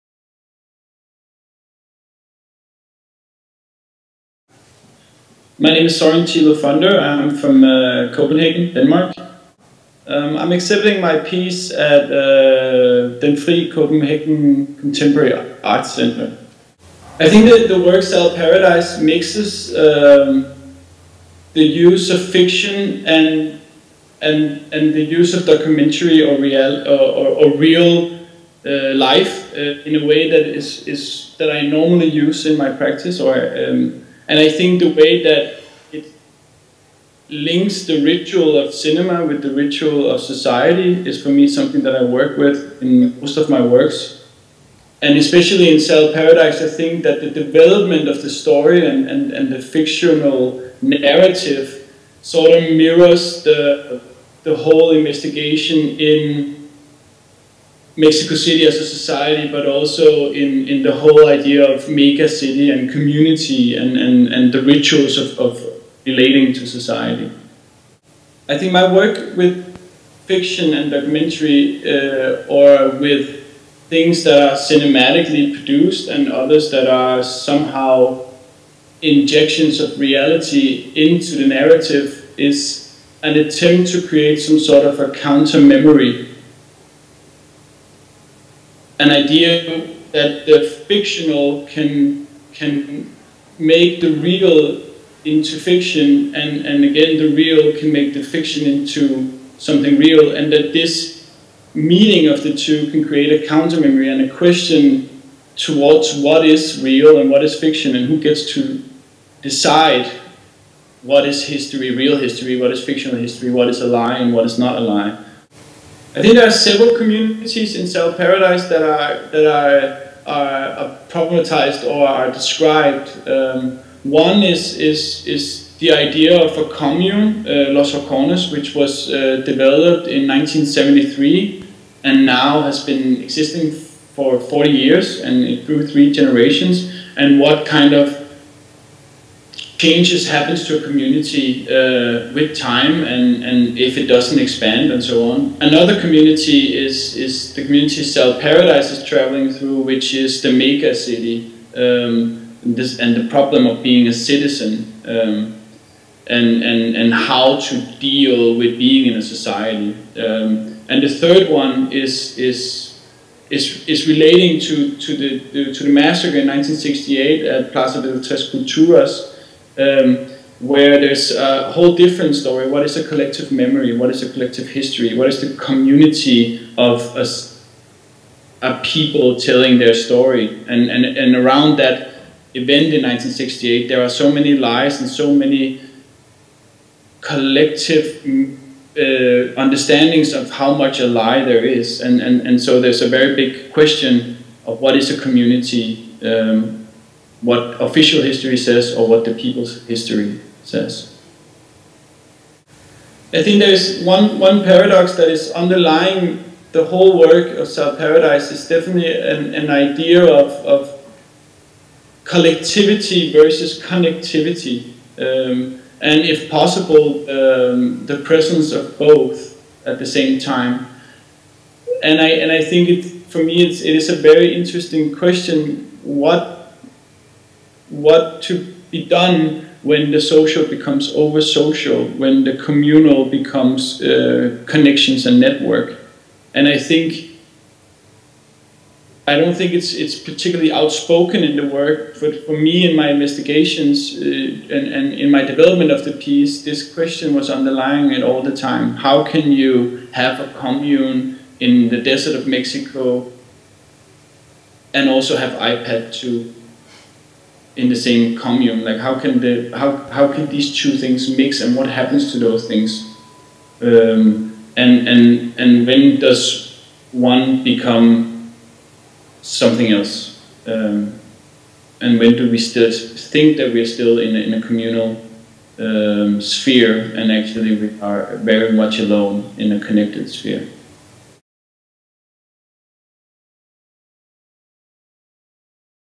INTERVIEW WITH SØREN THILO FUNDER -- COPENHAGEN ART FESTIVAL
Søren Thilo's video installation "Sal Paradise" is about experimental mini-communities in Mexico. It will be shown at Den Frie Centre of Contemporary Art.

Interviewed by: Anna Wistreich
Edited by: Jonas H. Christensen
The interview was recorded through Skype.